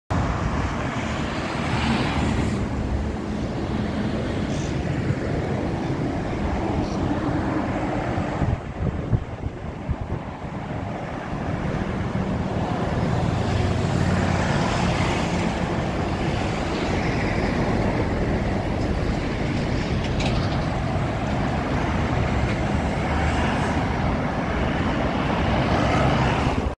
【国道16号】若柴交差点の歩道橋からの眺望【歩道橋】
国道16号若柴交差点の歩道橋からの眺望です。